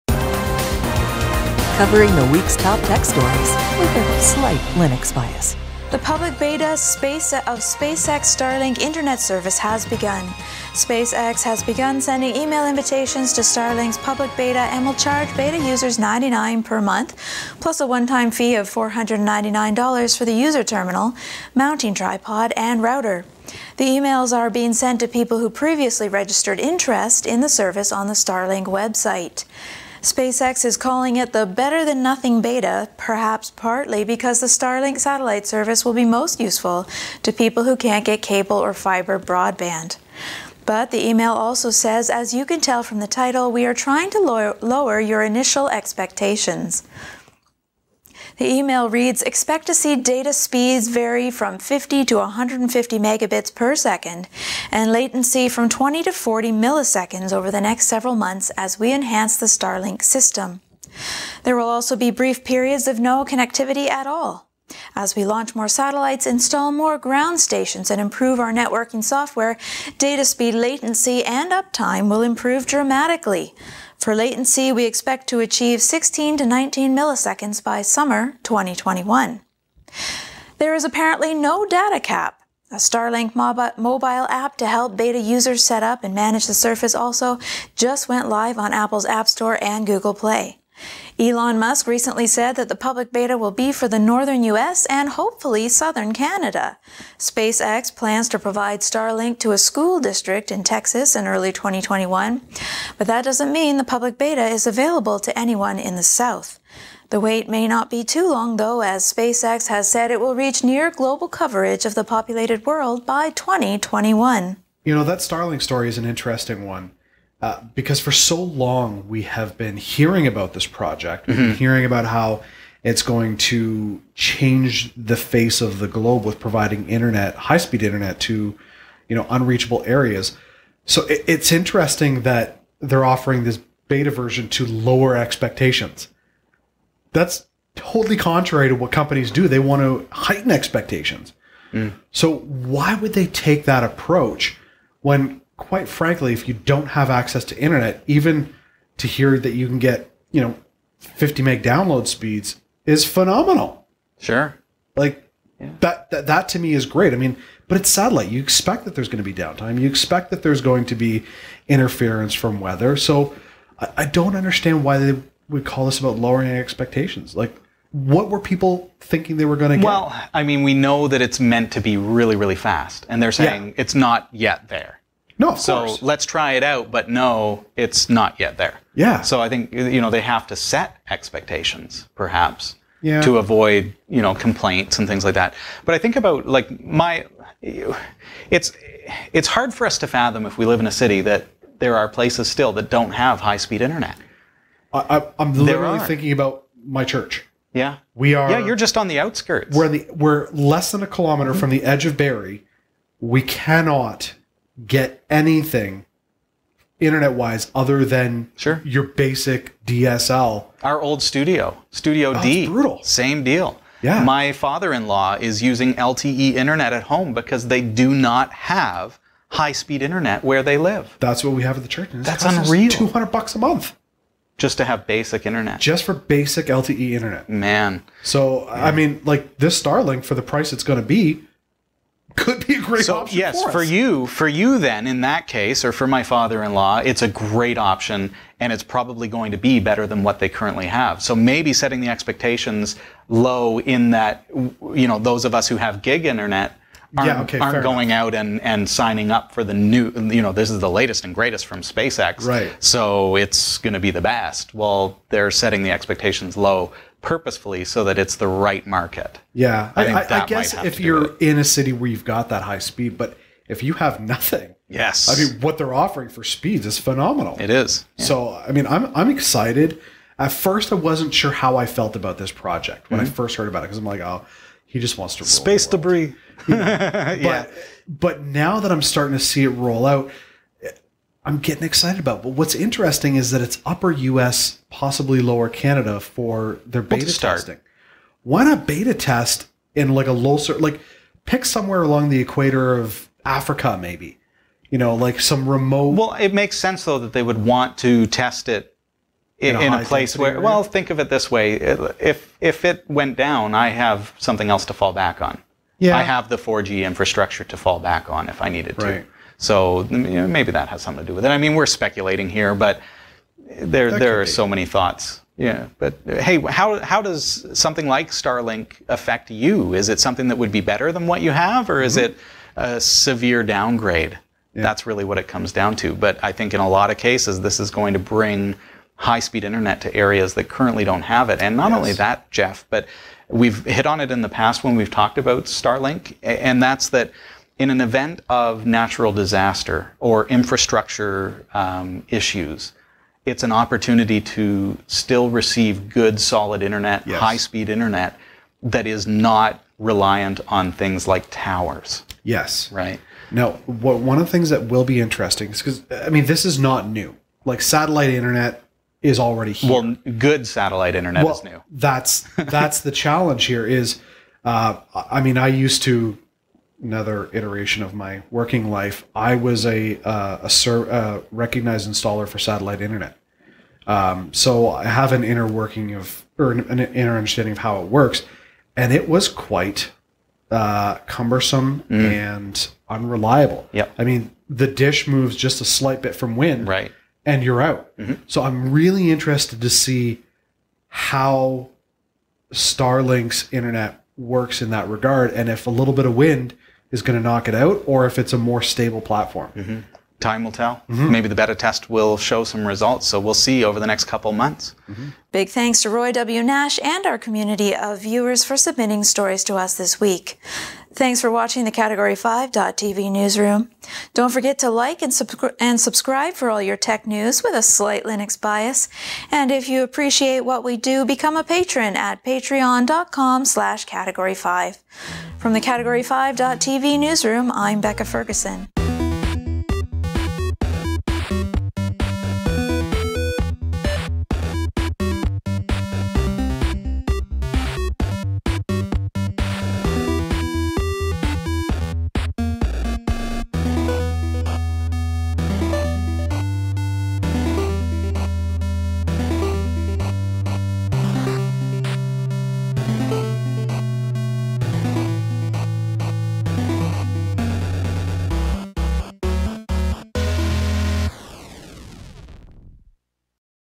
SpaceX Starlink Internet service beta has begun
SpaceX has begun sending email invitations to Starlink's public beta. The emails are being sent to people who previously registered interest in the service on the Starlink website, and they're trying to lower your initial expectations.